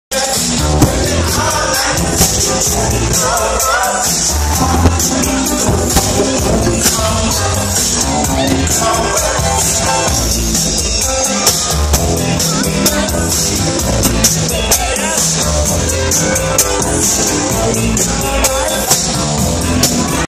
Noyce Wedding
Michael James & Auntie Kela working the soul train line